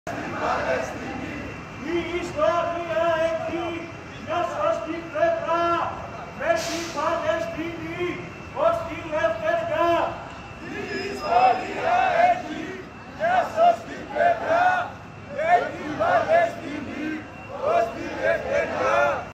Πανεκπαιδευτικό συλλαλητήριο στο Βόλο
Δυναμικές και μαζικές κινητοποιήσεις μαθητών και φοιτητών σήμερα σε Βόλο, Λάρισα και Τρίκαλα διεκδικώντας σχολεία και σπουδές που ν´ ανταποκρίνονται στις σύγχρονες ανάγκες τους ενώ με τα συνθήματα και τις σημαίες τους εξέφρασαν και την αλληλεγγύη τους στον Παλαιστινιακό λαό.

Στο Βόλο, μαζική κινητοποίηση μαθητών  και  φοιτητών έγινε μετά από κάλεσμα των φοιτητικών συλλόγων ΠΤΔΕ, ΙΑΚΑ, ΠΔΜΒ, Φυτικής παραγωγής, Μηχανολόγων μηχανικών, Αρχιτεκτονικής, Ηλεκτρολόγων μηχανικών και της Συντονιστικής Επιτροπής Μαθητών Βόλου.